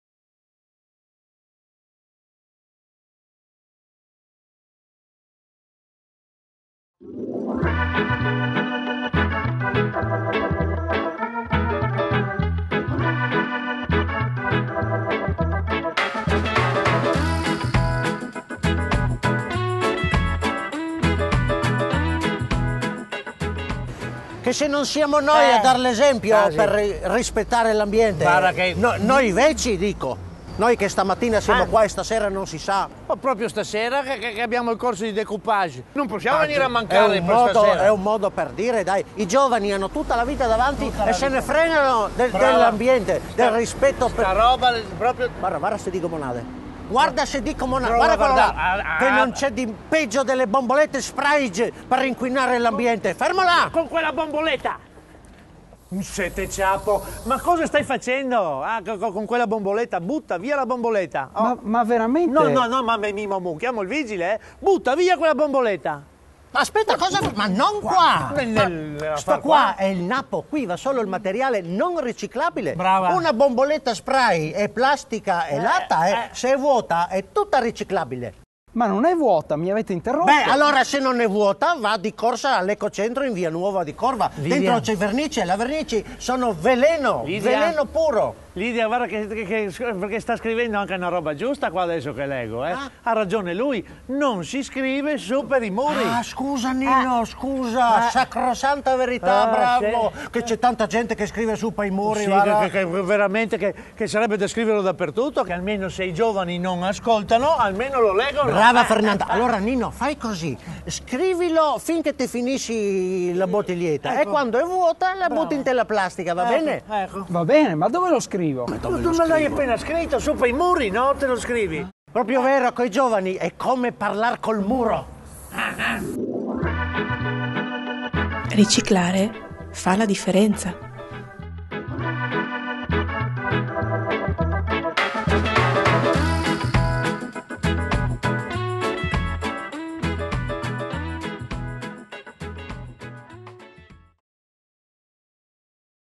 Impara a riciclare. "Non si scrive sui muri!"
Riciclare fa la differenza! I Papu, nelle vesti delle arzille Lidia e Fernanda, ci spiegano una volta per tutte come fare a Pordenone una raccolta differenziata senza errori.
In questa puntata, assieme ad un ospite speciale, ci spiegano cosa fare  cosa fare delle bombolette spray, quando sono vuote e quando non lo sono.
Spot del Comune di Pordenone realizzato in collaborazione con Pnbox.